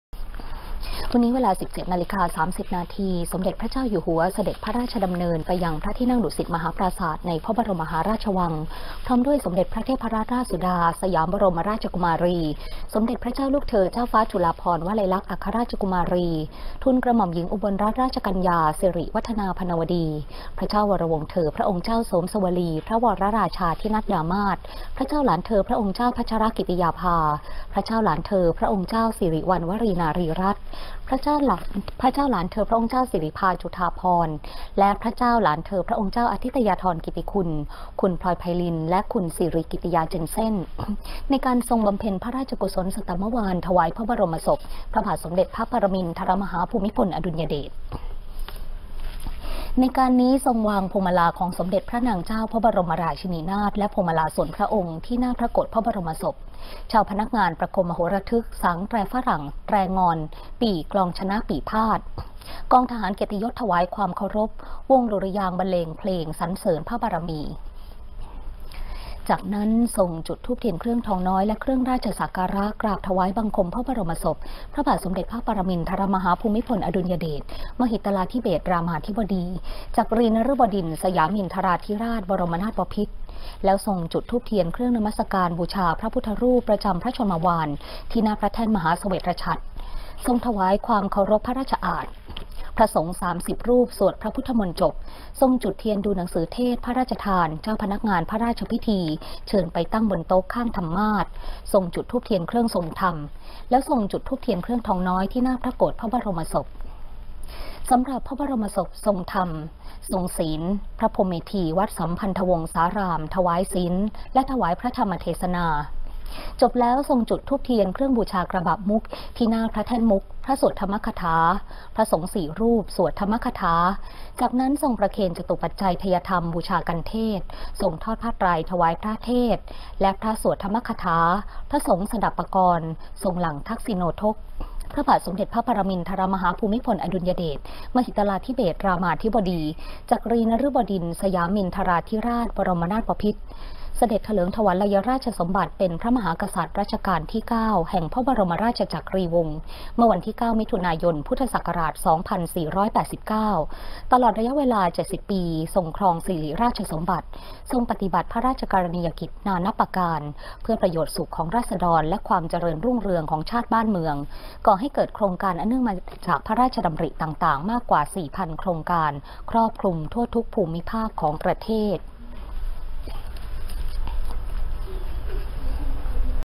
สมเด็จพระเจ้าอยู่หัว ทรงบำเพ็ญพระราชกุศล สตมวาร ๑๐๐ วัน เป็นวันที่ ๑
ออกอากาศเมื่อวันศุกร์ที่ ๒๐ มกราคม พ.ศ.๒๕๖๐